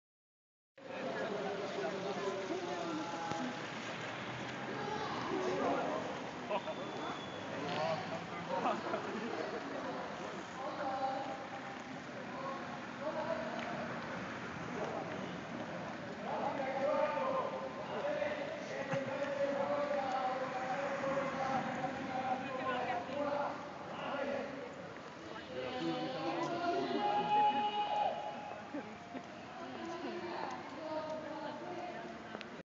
Partizánska Ľupča
Partizánska Ľupča (predtým Nemecká Ľupča) je liptovská obec na Slovensku v okrese Liptovský Mikuláš. Kostol sv. Matúša, obecný úrad, námestie.